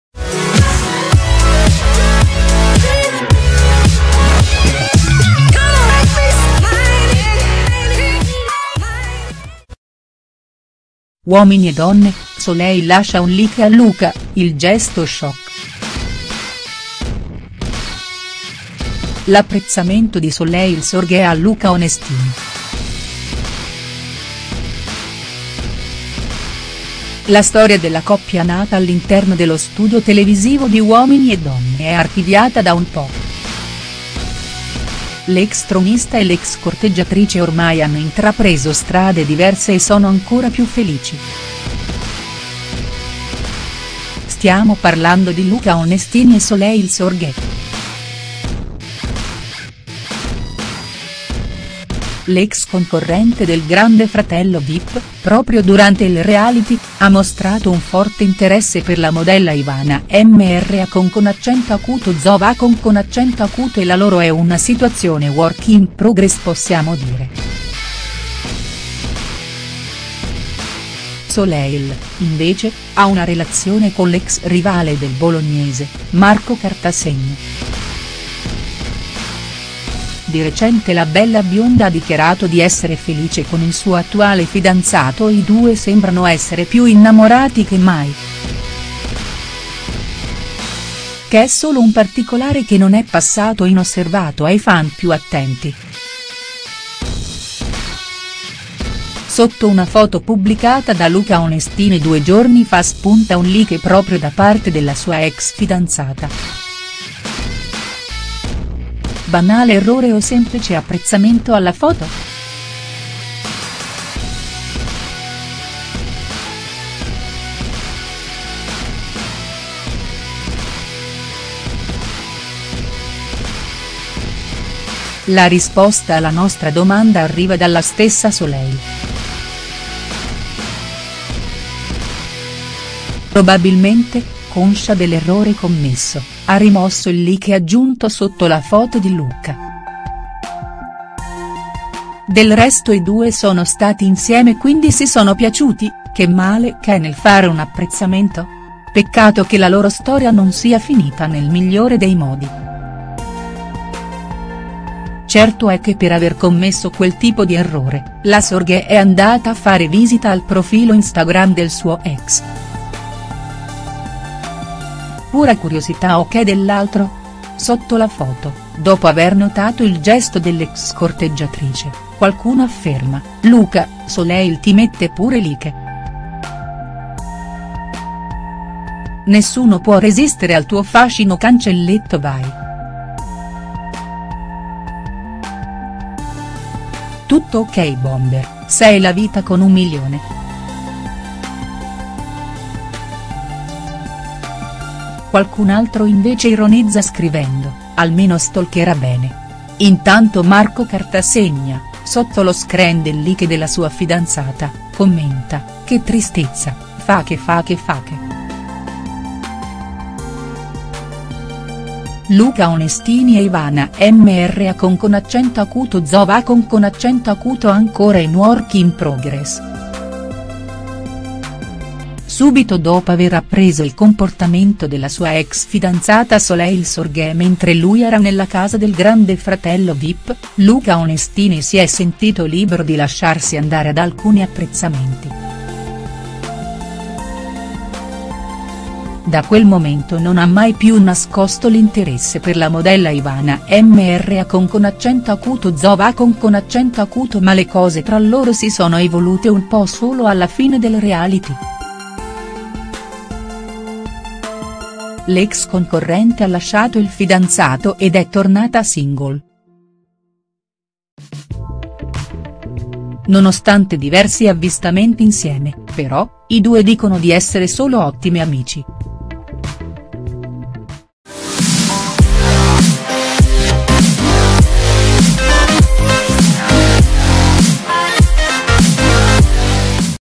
Uomini e Donne, Soleil lascia un like a Luca: il gesto shock| STARS NEWS
Dopo la fine della relazione con Luca Onestini, spunta un like (poi rimosso) ad una foto dell'ex tronista.